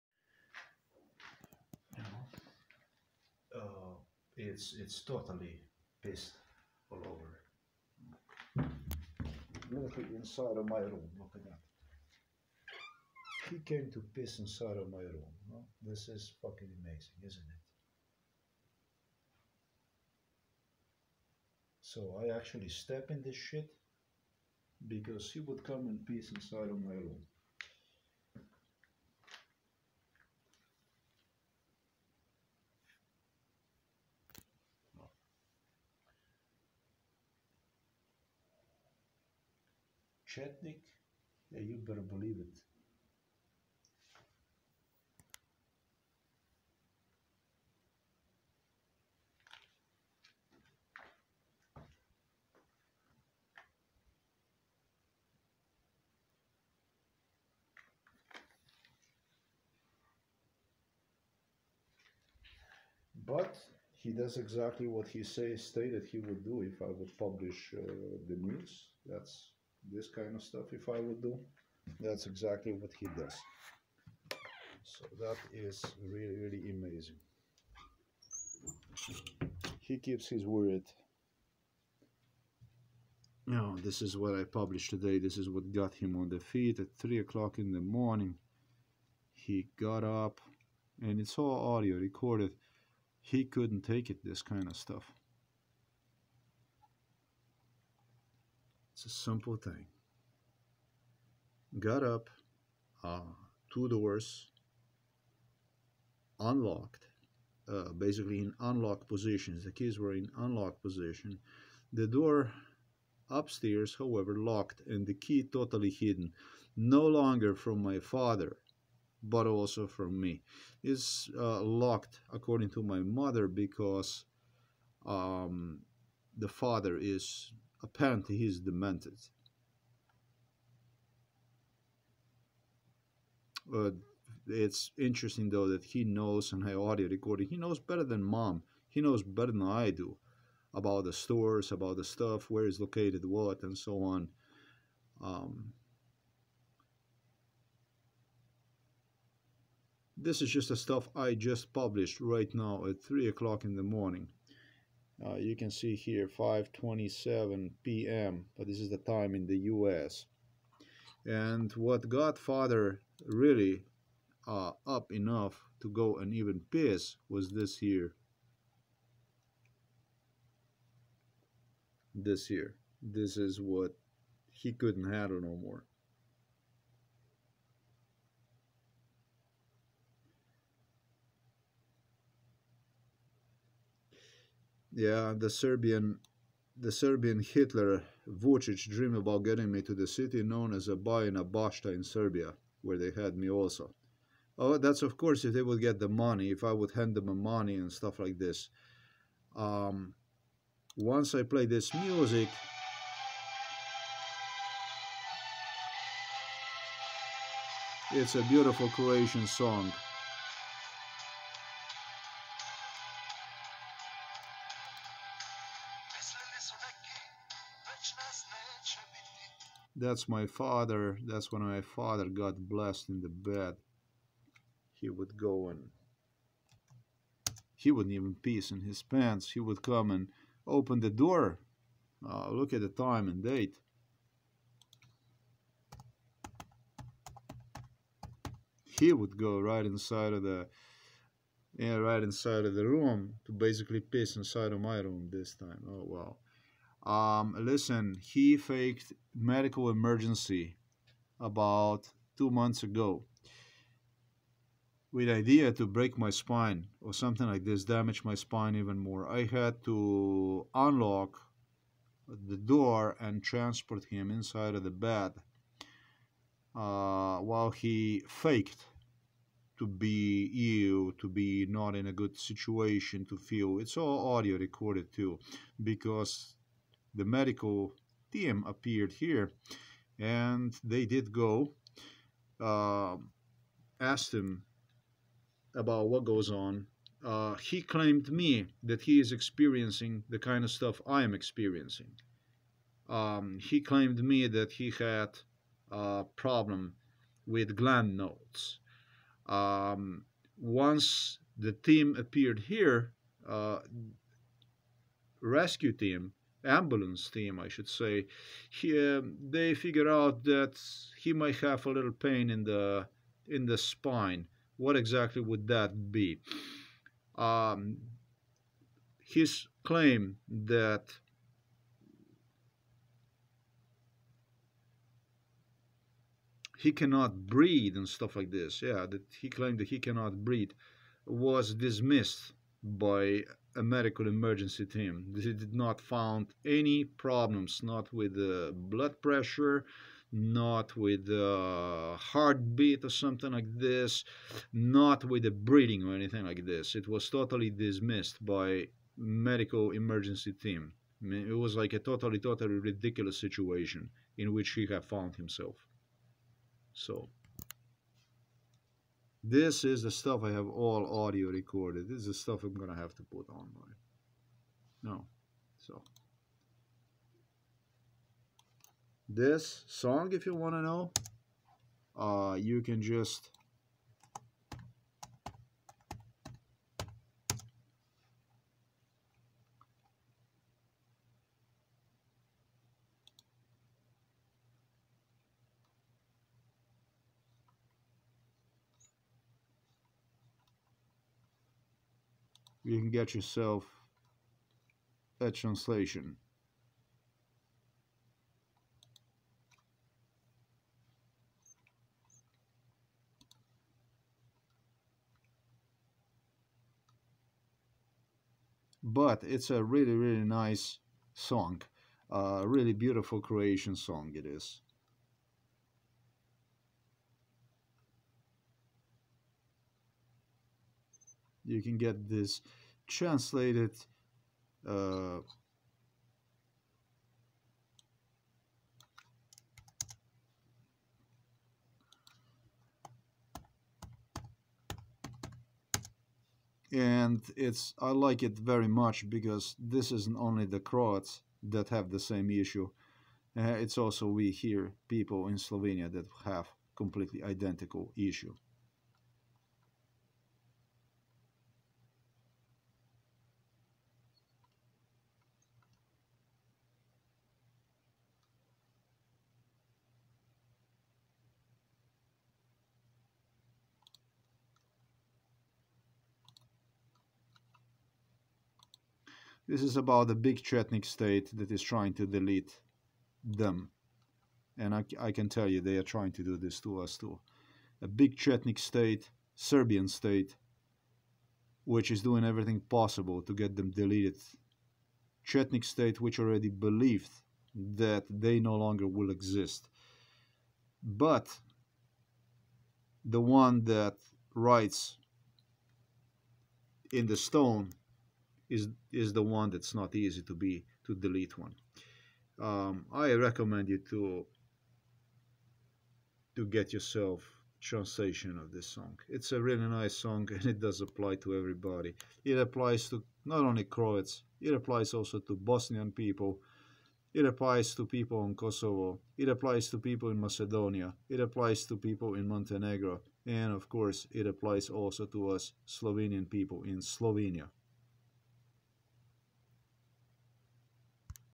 Father goes on to protest my news into my room via pissing spree at 0300 am
Father goes on to protest my news into my room via pissing spree at 0300 am. He then denied seen on video while washing himself inside of the bathroom.